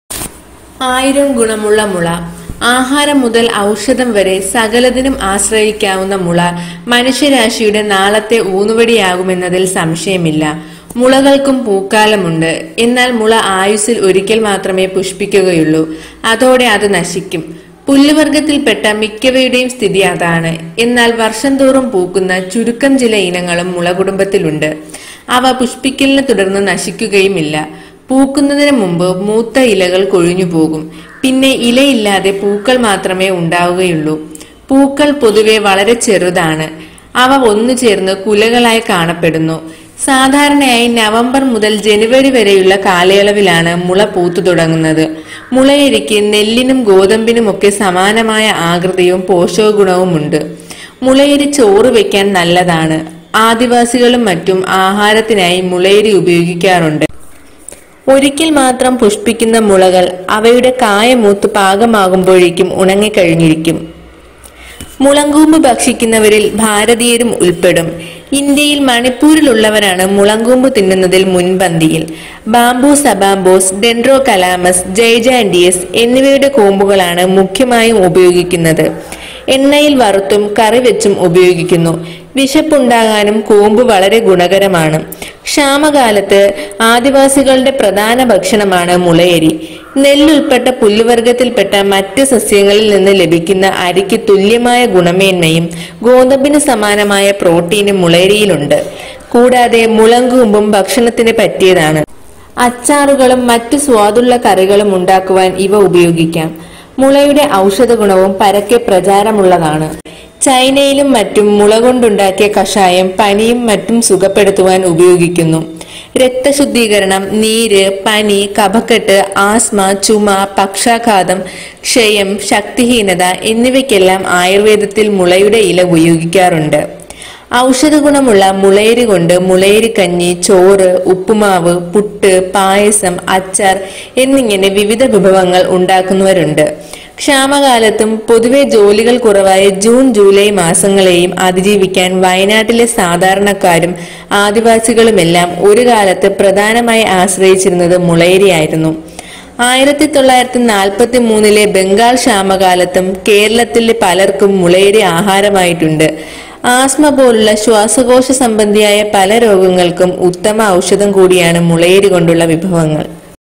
life tips about bamboo ayurveda
നമ്മൾക്ക് എല്ലാർക്കും പരിചിതമായി ഈ മുളയുടെ ആയുർവേദ ഗുണങ്ങൾ ഒന്ന് അറിഞ്ഞിരിക്കേണ്ടത് തന്നെയാണ്